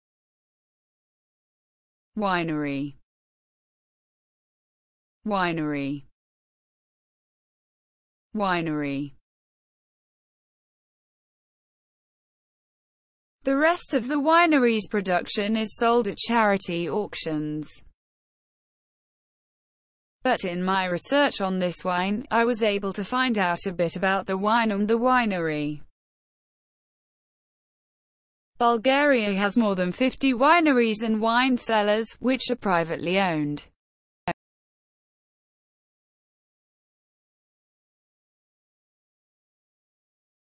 winery: How to pronounce winery with Phonetic and Examples
This video shows how to pronounce winery, winery meaning, winery definition, winery phonetic, winery synonyms and winery example

winery Definition : an establishment where wine is made
winery Phonetic : 'wain ri
winery Examples :
The rest of the winery's production is sold at charity auctions.
But in my research on this wine, I was able to find out a bit about the wine and the winery.
Bulgaria has more than 50 wineries and wine cellars, which are privately owned.